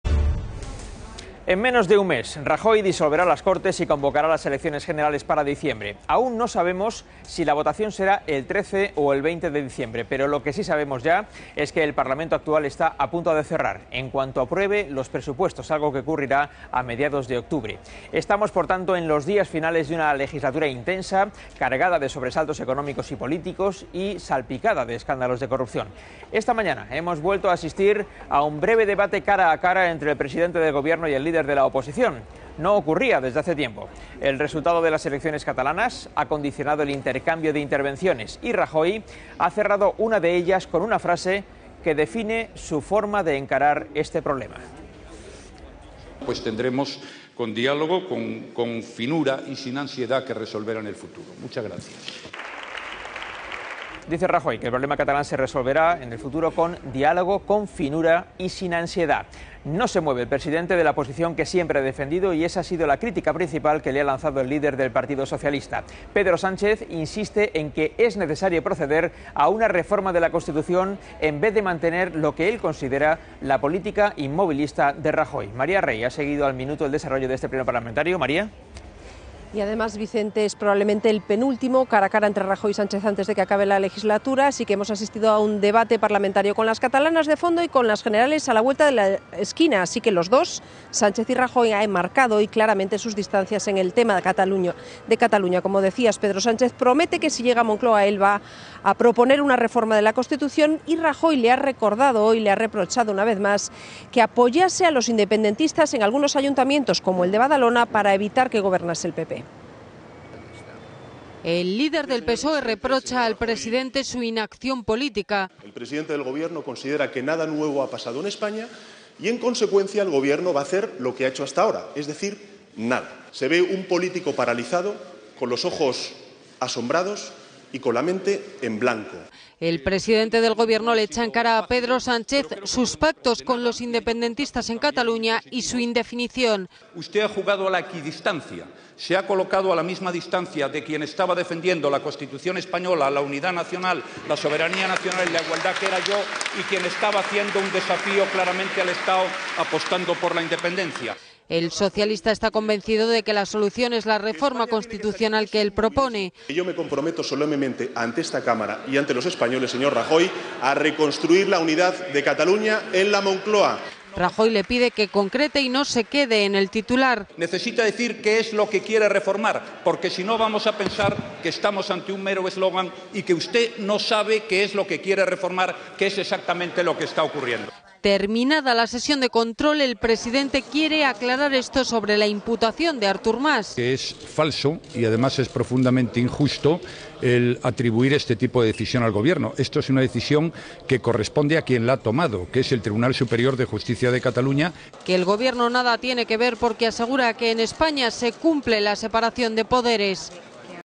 Rajoy reprocha a Sánchez que "juega con la equidistancia" ante los separatistas
Rajoy ha criticado la postura de Sánchez y le ha emplazado a concretar la reforma constitucional que defienden los socialistas para "no pensar que se está ante un mero eslogan".